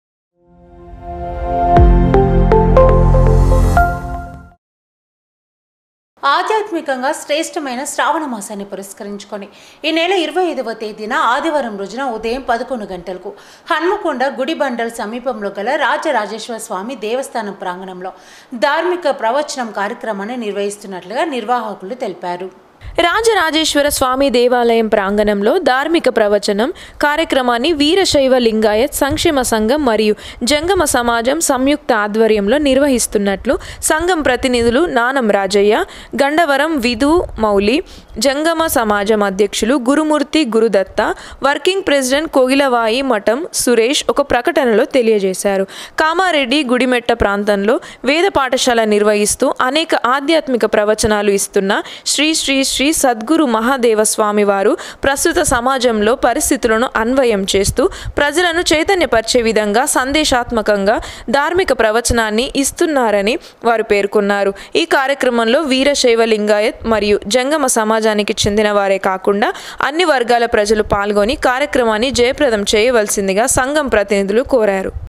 హనుమకొండ గుడిబండల్లోని రాజరాజేశ్వర స్వామి దేవస్థానంలోధార్మిక ప్రవచనం కార్యక్రమం నిర్వహించనున్నారు
ఆధ్యాత్మికంగా శ్రేష్టమైన శ్రావణమాసం పురస్కరించుకొని 25వ తేదీన ఆదివారం రోజున ఉదయం 11 గంటలకు హన్మకొండ గుడిబండల్ సమీపంలో గల రాజరాజేశ్వర స్వామి దేవస్థానం ప్రాంగణంలో ధార్మిక ప్రవచనం కార్యక్రమం నిర్వహిస్తున్నట్లుగా నిర్వాహకులు తెలిపారు